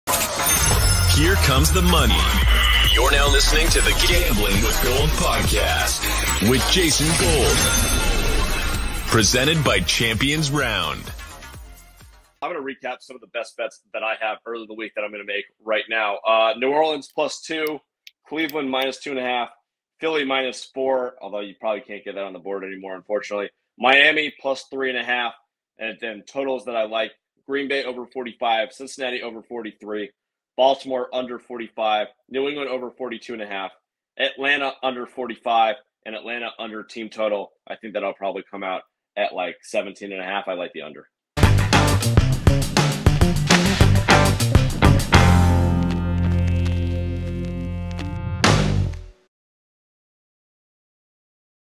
Early Week 6 BEST Bets | Gambling With Gold
Early NFL Week 6 best bets, totals and predictions.